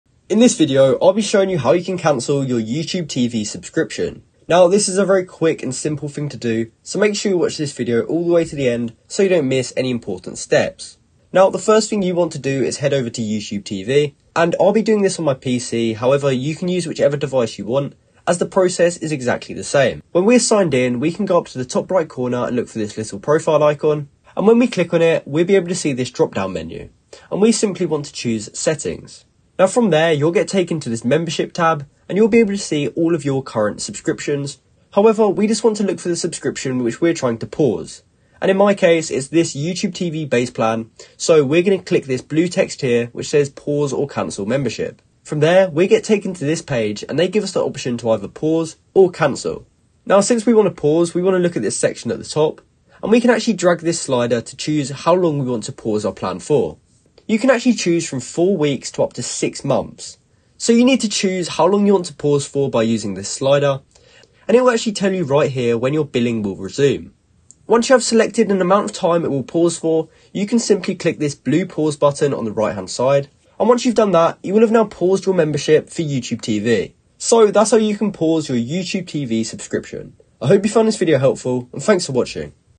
How To Pause Youtube TV Subscription (2024)
In today's video i will cover how to pause youtube tv subscription. This will show you how to pause or cancel youtube tv as you can also cancel youtube tv on the same page as the pause youtube tv subscription page!